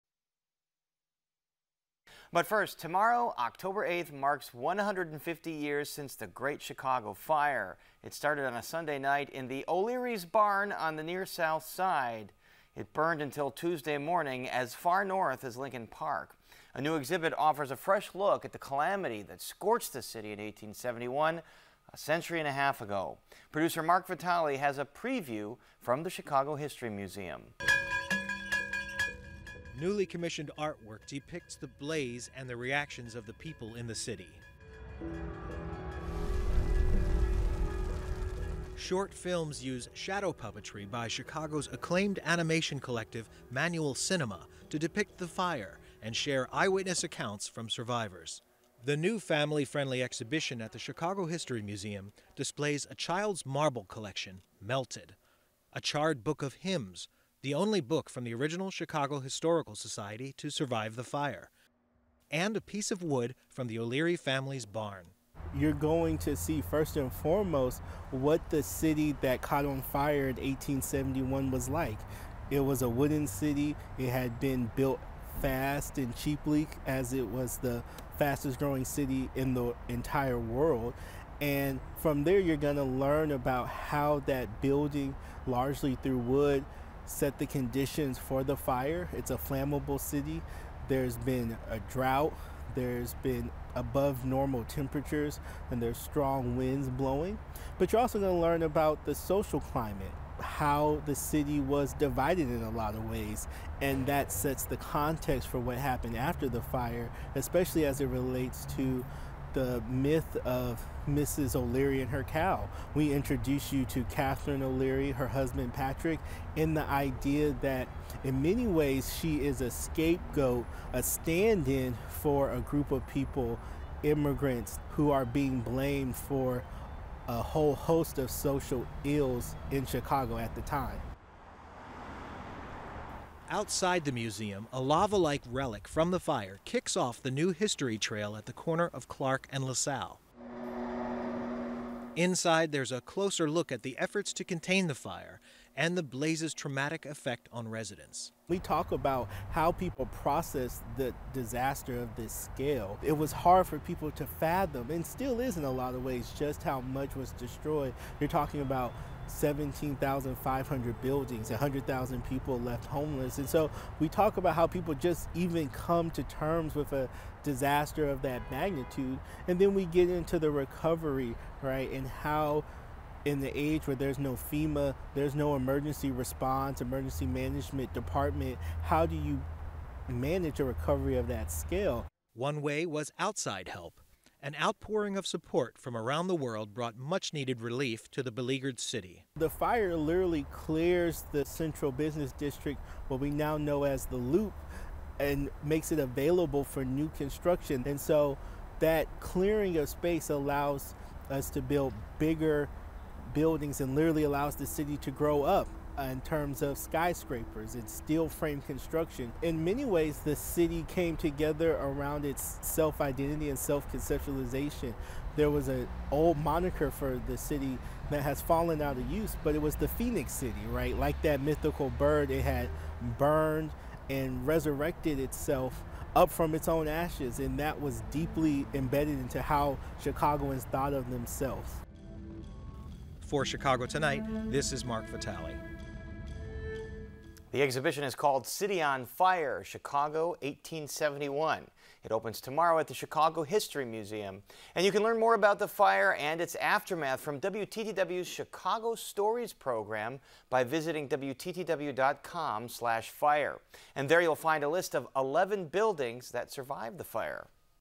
Chicago History Museum Remembers Great Fire of 1871
October 8, marks 150 years since the start of the Great Chicago Fire. It started on a Sunday night in the O’Leary’s barn on the near South Side. It burned until Tuesday morning as far north as Lincoln Park. A new exhibit offers a fresh look at the calamity that scorched the city in 1871, a century and a half ago.